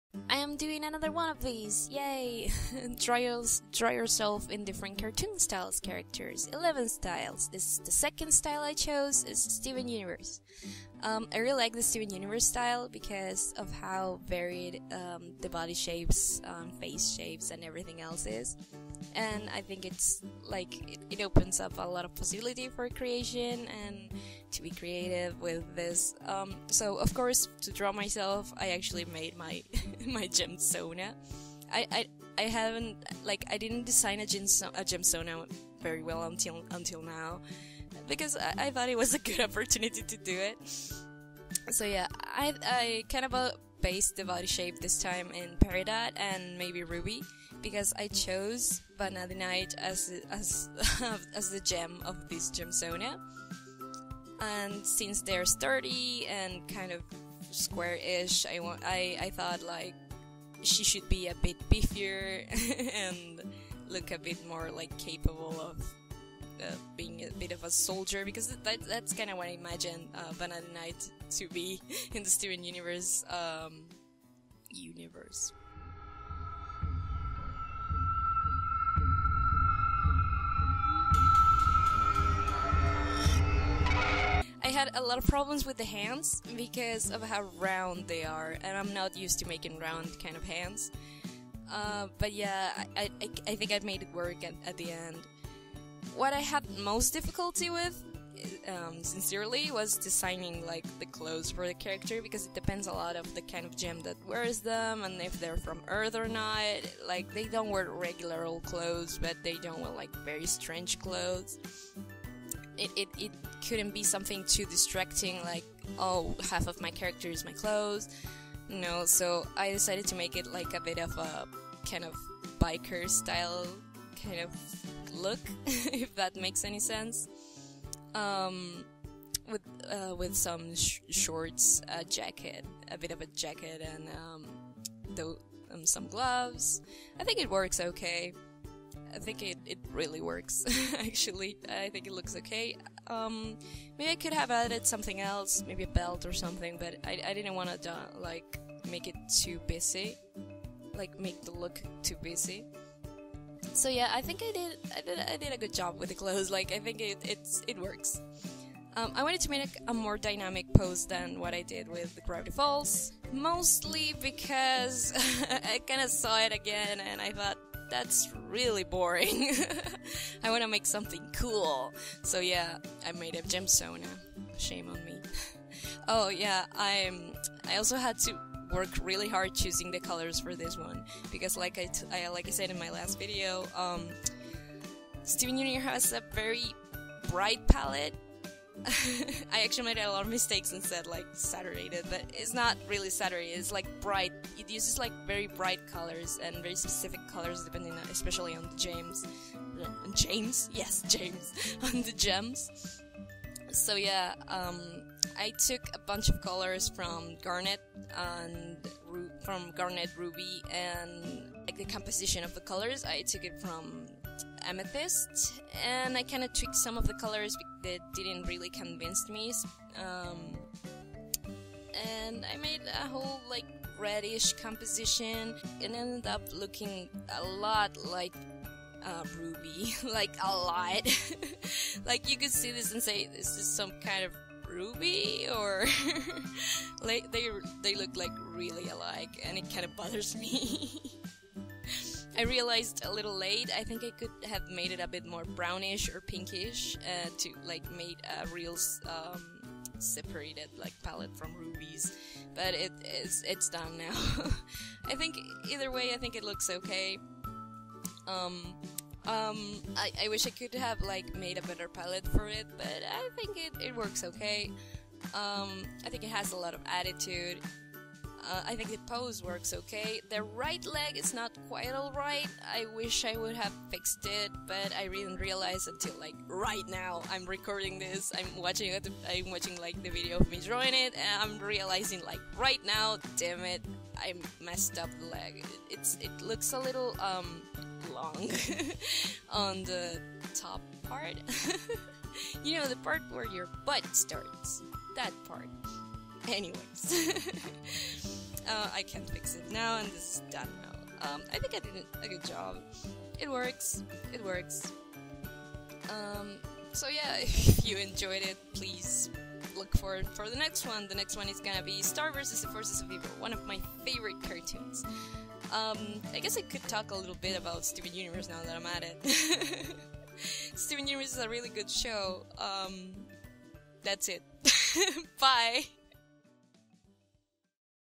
Draw yourself STEVEN UNIVERSE STYLE
A little project I am working on

Drawing myself in 11 cartoon styles!

Forgive my english, my native tongue is spanish.

Feel free to correct me.